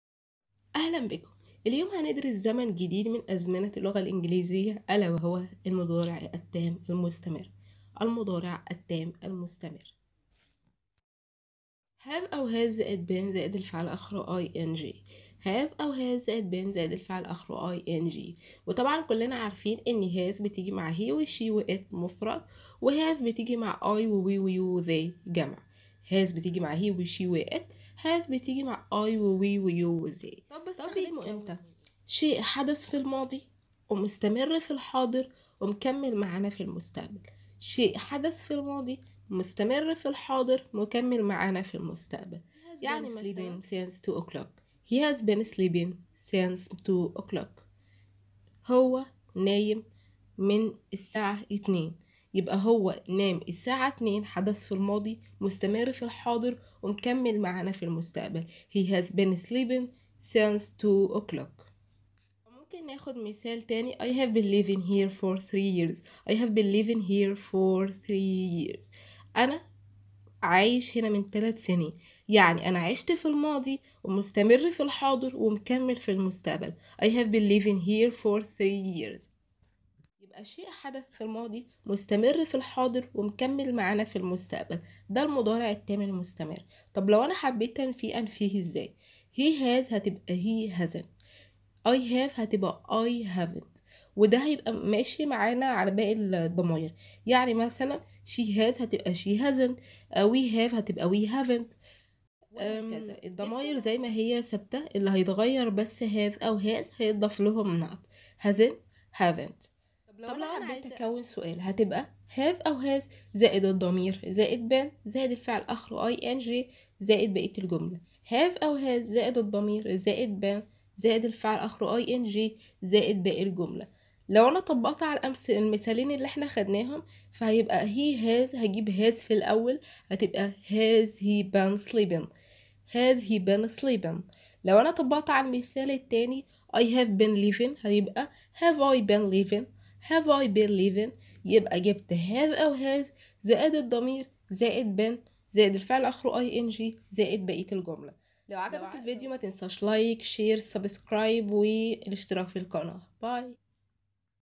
شرح زمن المضارع التام المستمر فى اللغة الانجليزية The present perfect continuous
تعلم معنا اللغة الانجليزية
شرح زمن المضارع التام المستمر فى اللغة الانجليزية
The present perfect continuous

Has . Have + been + v + ing